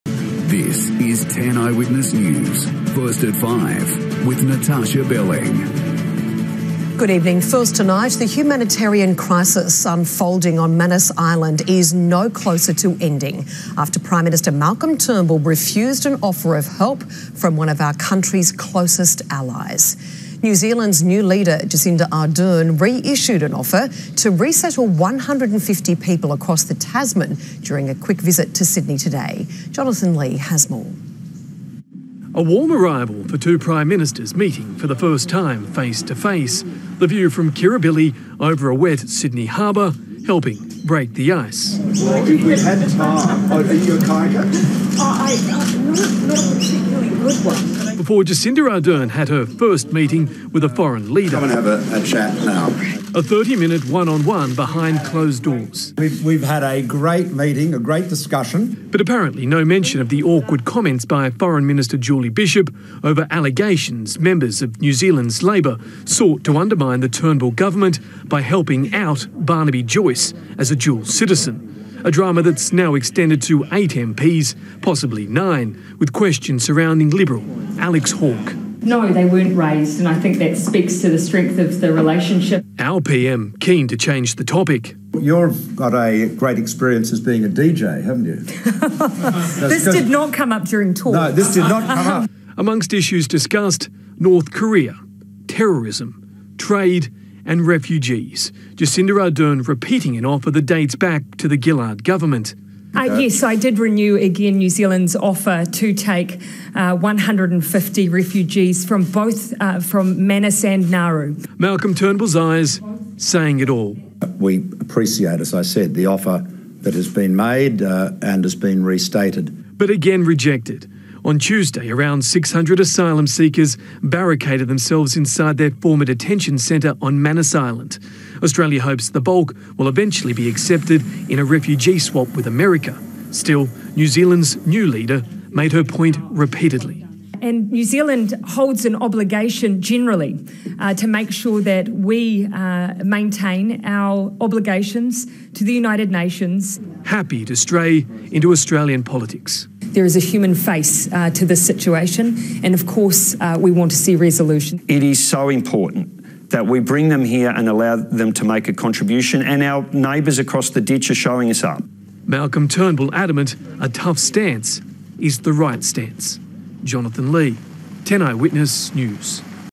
PM Malcolm Turnbull refuses offer from PM Jacinda Ardern to assist refugees
5/11/2017 New Zealand PM Jacinda Ardern extends to Australia an offer to take 150 refugees from Manus and Nauru.